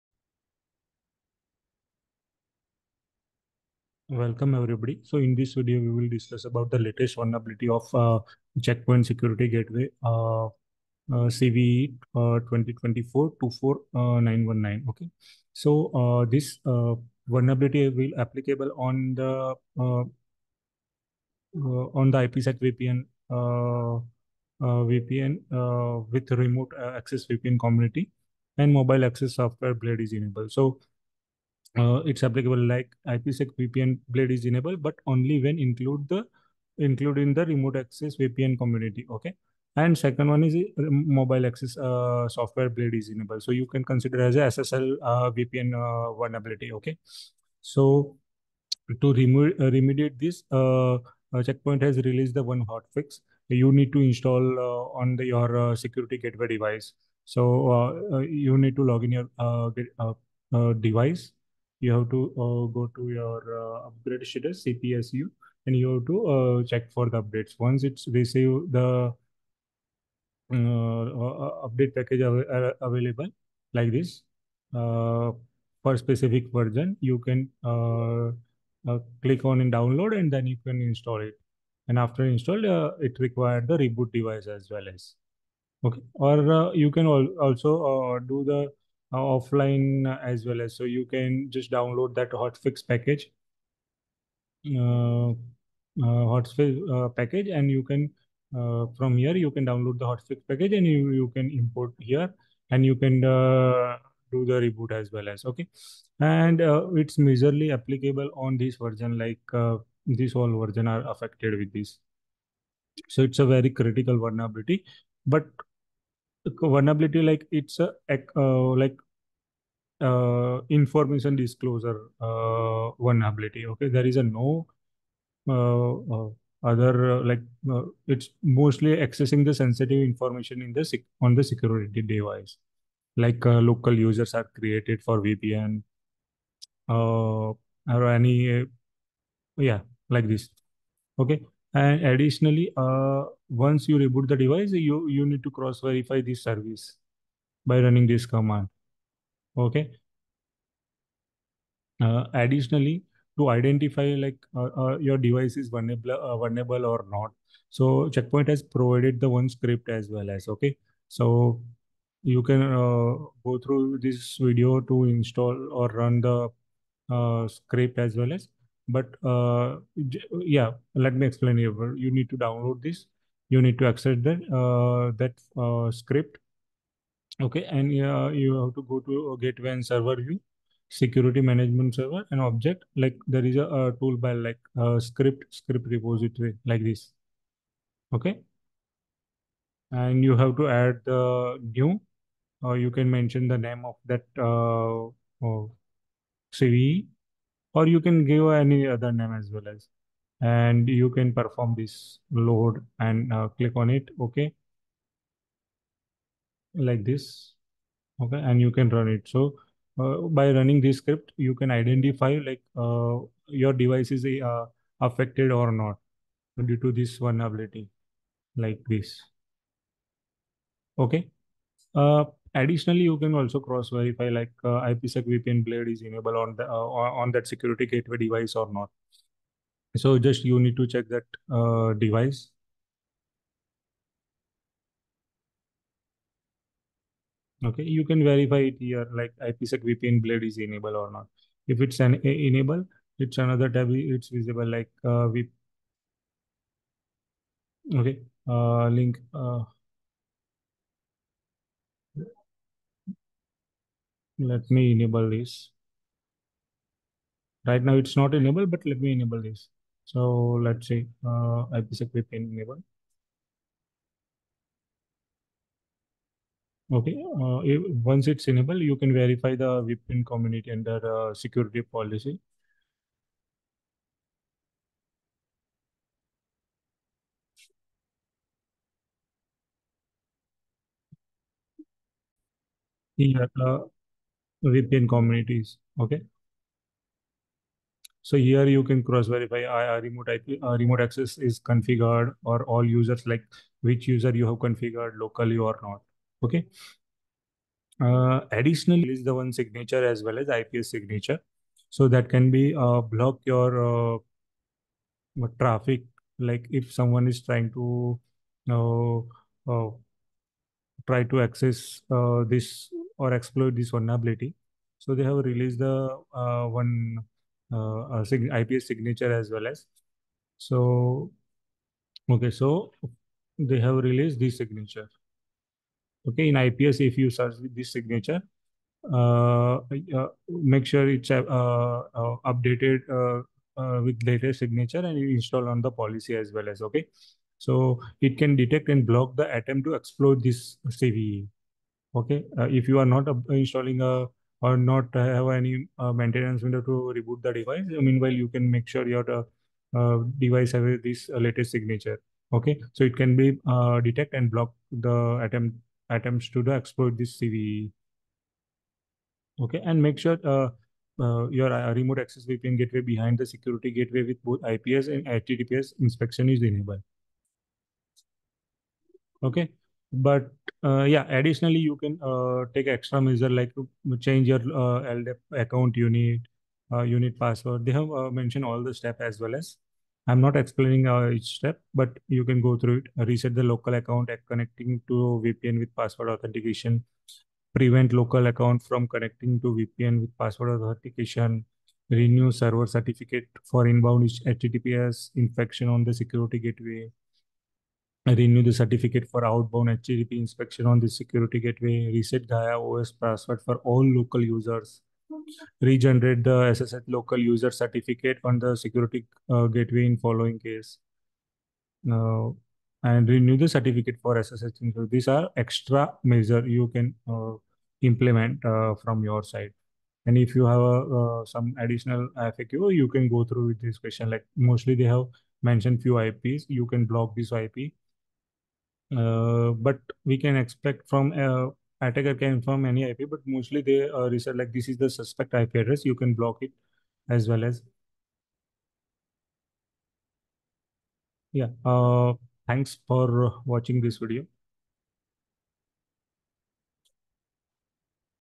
CVE-2024-24919: Check Point Security Gateway Information Disclosure Zero-Day Exploited in the Wild
Check Point’s dedicated task force continues investigating attempts to gain unauthorized access to VPN products used by our customers. On May 28, 2024 we discovered a vulnerability in Security Gateways with IPsec VPN in Remote Access VPN community and the Mobile Access software blade (CVE-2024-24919). Exploiting this vulnerability can result in accessing sensitive information on the Security Gateway.
This, in certain scenarios, can potentially lead the attacker to move laterally and gain domain admin privileges.

The IPS Signature "Check Point VPN Information Disclosure (CVE-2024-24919)" detects and blocks attempts to exploit this CVE.
This signature is automatically available in the "Optimized" IPS profile.

To prevent any attempt to exploit this vulnerability, you must protect the vulnerable Remote Access VPN gateway behind a Security Gateway with both IPS and HTTPS Inspection enabled.

Important extra measures

1. Change the password of the LDAP Account Unit
2. Reset password of local accounts connecting to VPN with password authentication
3. Prevent Local Accounts from connecting to VPN with Password Authentication
4. Renew the server certificates for the Inbound HTTPS Inspection on the Security Gateway
5. Renew the certificate for the Outbound HTTPS Inspection on the Security Gateway
6. Reset Gaia OS passwords for all local users
7. Regenerate the SSH local user certificate on the Security Gateway in the following case:
8. Renew the certificate for the SSH Inspection

deploy the hotfix for CVE-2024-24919 to address the vulnerability.

Implement one of the additional protection measures If you have remote access users who authenticate to the Security Gateway using only a password:

Reset passwords for local users.
Prevent local users from authenticating with VPN.